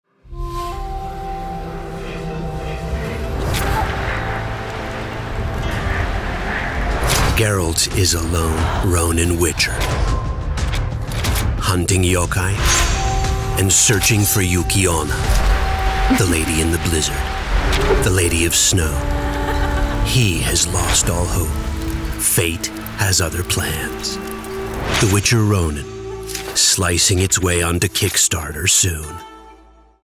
The Witcher: Ronin Comic Trailer
Presenting The Witcher: Ronin — an original comic set in an ancient-Japanese-inspired world where monster slayer Geralt trails the mythical Lady of Snow Yuki Onna.

Interested? Follow the KickStarter page to be up to date when we launch the campaign